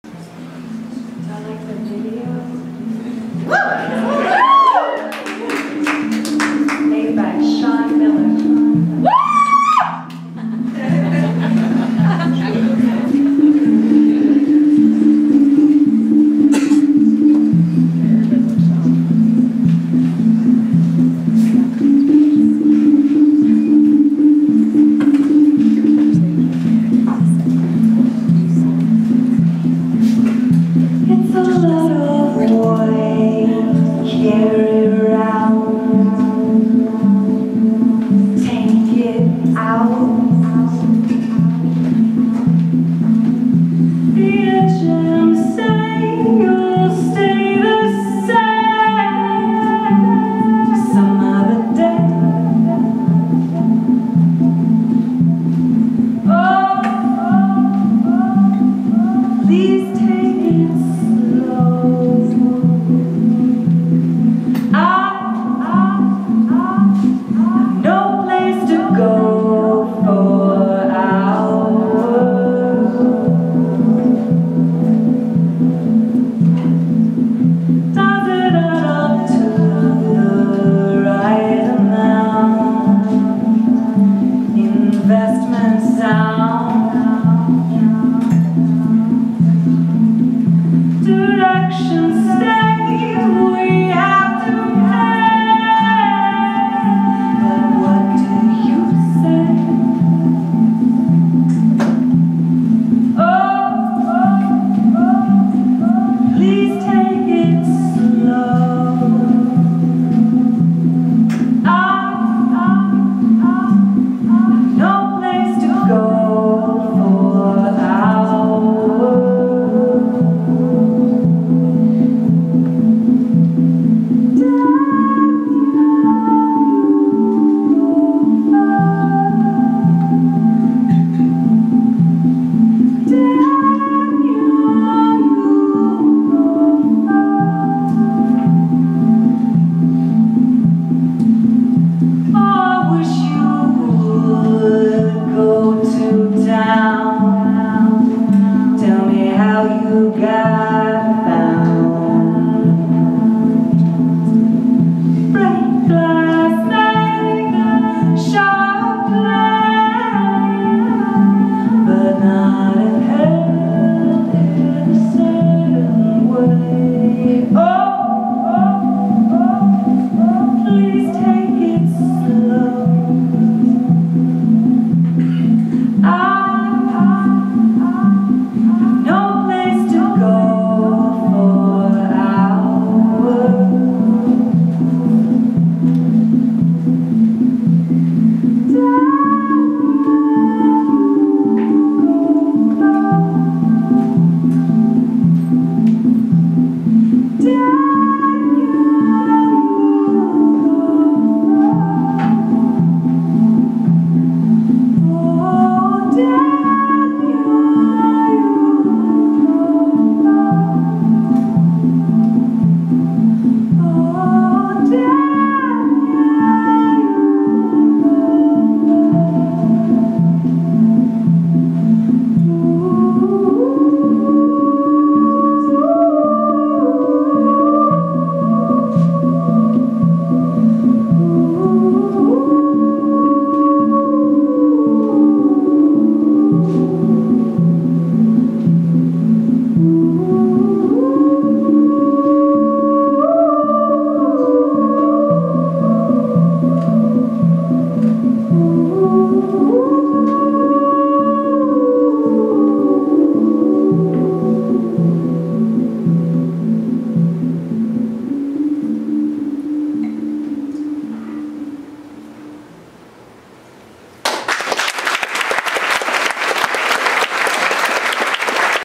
Lily Taylor at The Kessler Theater in Dallas, Texas
Recorded live on June 21, 2015 at the historic Kessler Theater in X+/North Oak Cliff